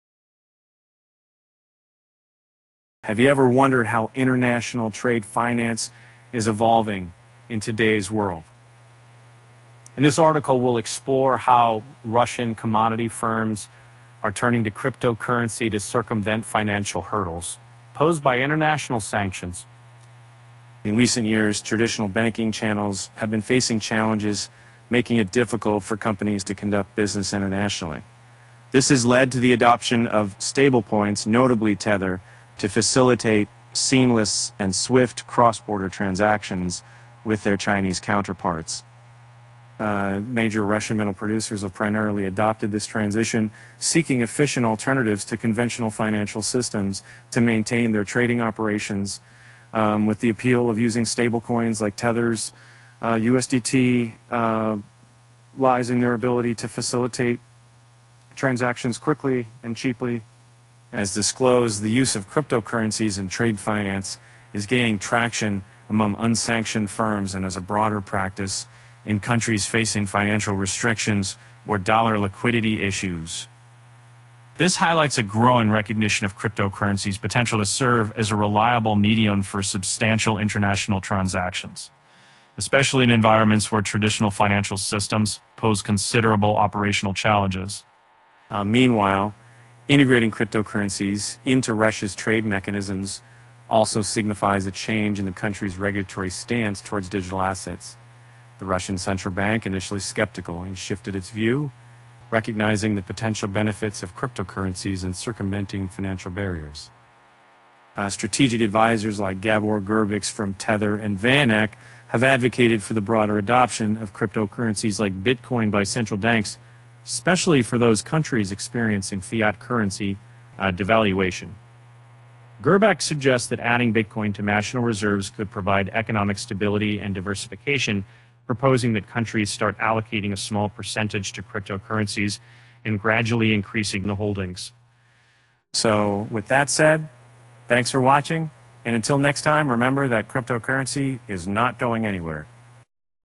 Russian Firms Pivot To Crypto In Global Trade Amid Sanctions – Here’s How
Have you ever wondered how international trade finance is evolving in today's world? In this article, we'll explore how Russian commodity firms are turning to cryptocurrency to circumvent financial hurdles posed by international sanctions.